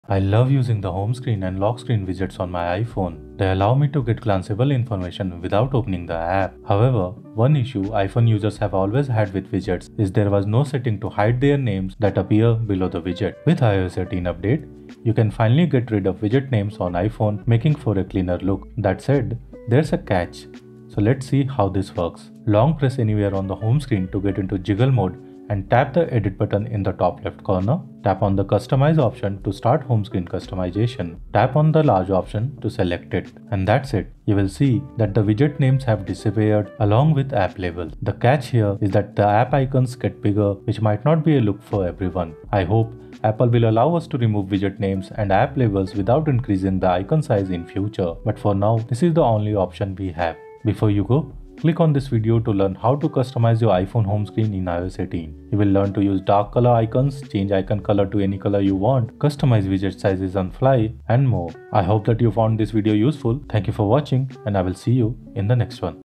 How to Get Rid of Widget Names on iPhone
Here's how to remove widget names on your iPhone Home Screen with iOS 18 update. Get rid of widget names on iPhone.

Purchases made via our links may earn us commission.

🎥 Our Kit and tools:

Camera Sony a6700
Lens (Sigma 23mm f1.4)
Wireless charging station
USB-C Docking Station